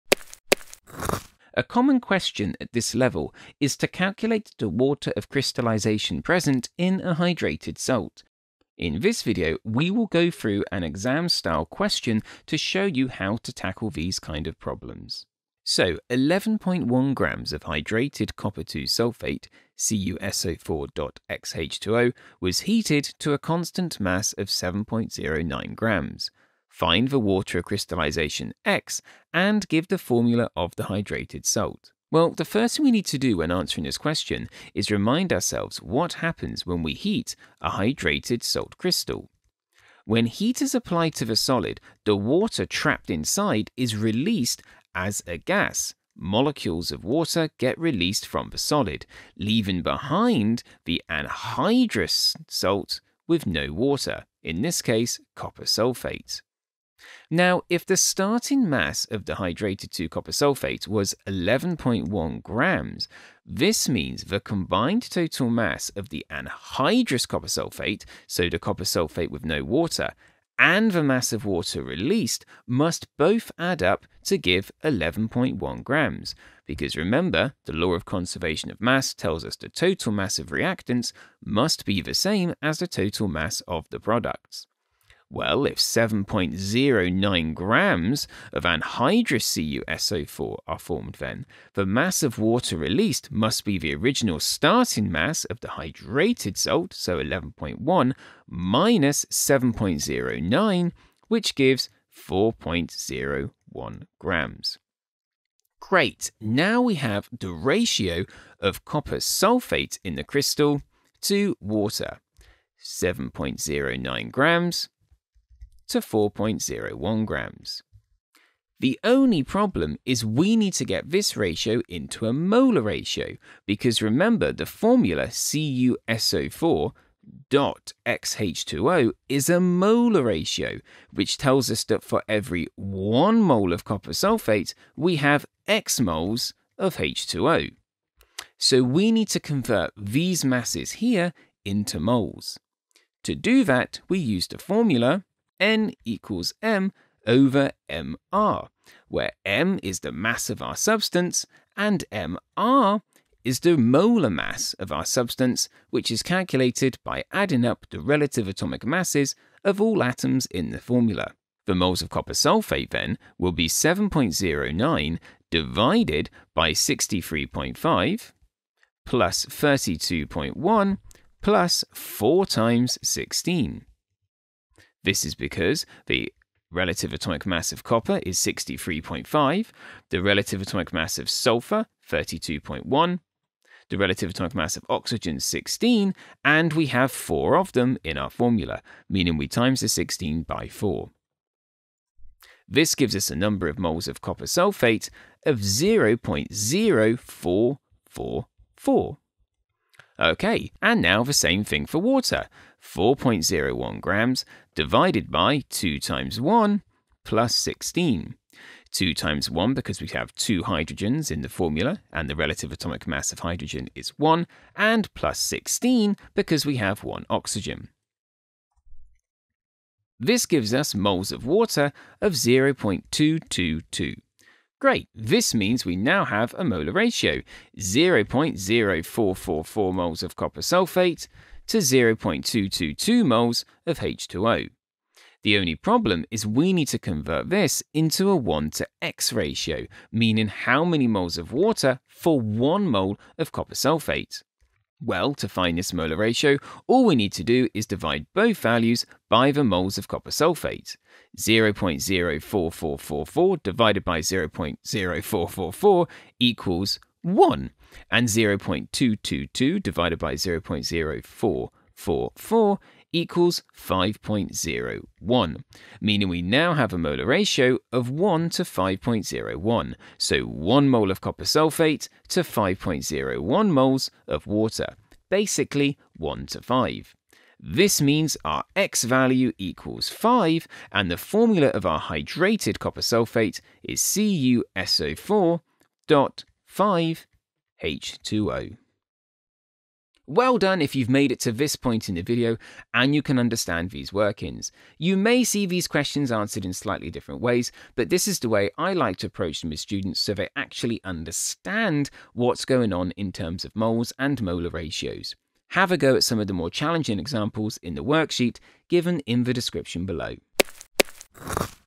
Water of Crystallisation: Worked Solution to Exam-Style Question
In this video, we tackle a common exam question on calculating the water of crystallisation in a hydrated salt. Using copper(II) sulfate as our example, we go through each step to find the formula of the hydrated salt, CuSO₄·xH₂O. Perfect for all A-level exam boards and IB Chemistry students, this video will help you understand the method for determining the value of "x" with a clear, worked solution.

Please note - during the video values used in workings aren’t rounded - to keep workings easier to follow the first 3 significant figures are written (0.222 moles of water instead of 0.2227… for example) but the values used in further calculations aren’t rounded. Remember to only round values at the very end for your final answer - carry all values through without rounding.